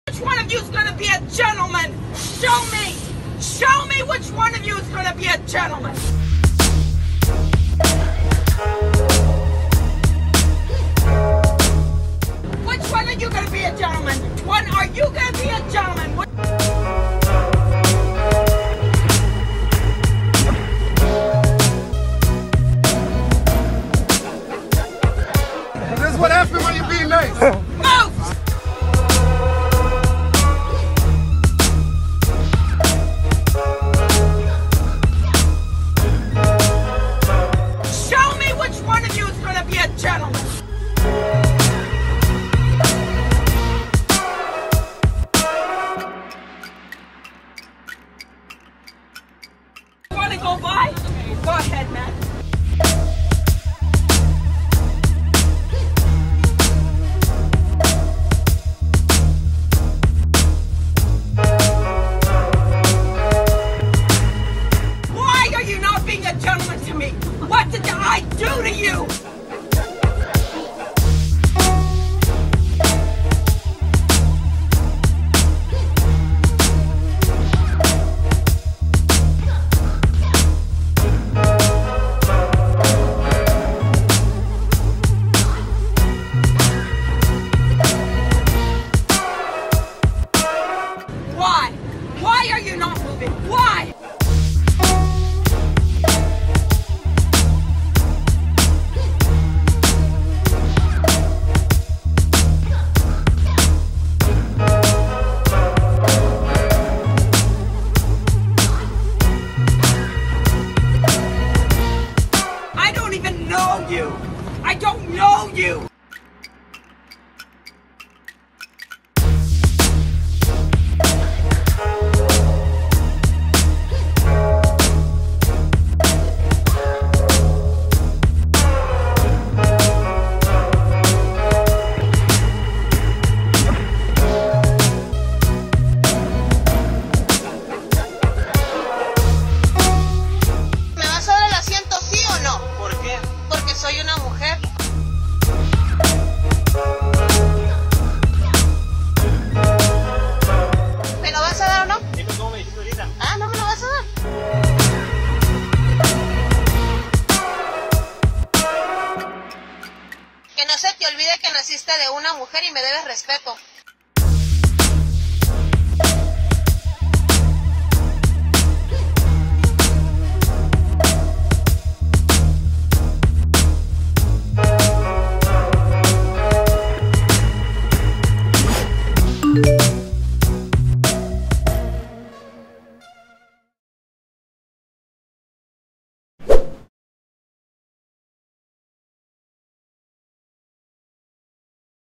Señora pierde la cabeza por un asiento en el transporte público
Una señora perdió la cabeza al ver que nadie le cedía el asiento en el camión y a pesar de que no era la única que iba de pie, haría todo por conseguir un lugar. Durante todo su trayecto no dejó de insultar y gritarles a los pasajeros, pero al ver que nadie le hacía caso… haría algo más descabellado, todo por un asiento “Estas señoras son un claro ejemplo de cómo no es el feminismo… Por favor no las asocien con nosotras”